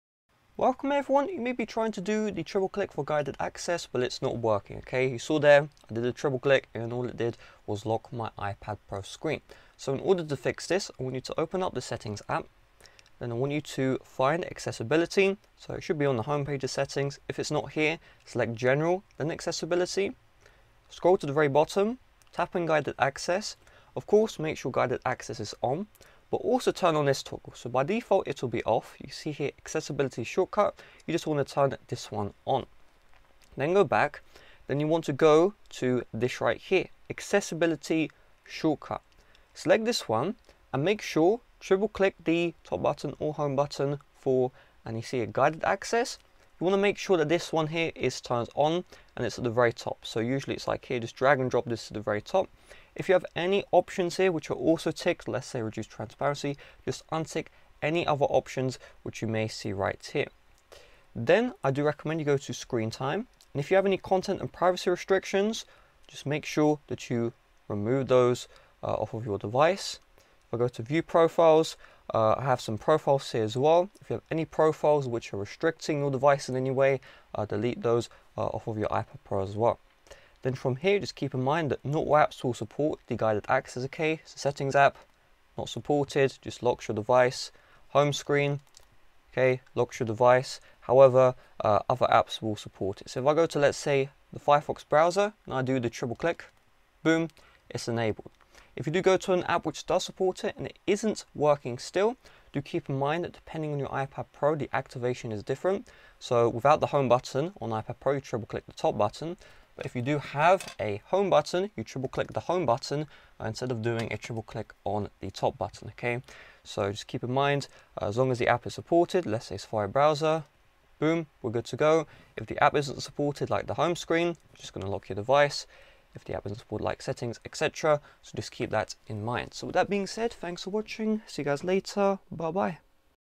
Guided Access Triple Click NOT Working? [SOLVED]
Guided Access Triple Click NOT Working | FIX iPhone, iPad, iPad Air & iPad Pro which does not have guided access working! Do you own an iPhone, iPhone Pro, iPhone mini, iPad, iPad Pro, iPad air and when trying to activate guided access it is not working? If so, this video is for you! I will teach you how to fix guided access triple click not working in this video. By the end of this guide, you will have enabled guided access without fail, and after doing a triple click you won't be brought to the lock screen anymore.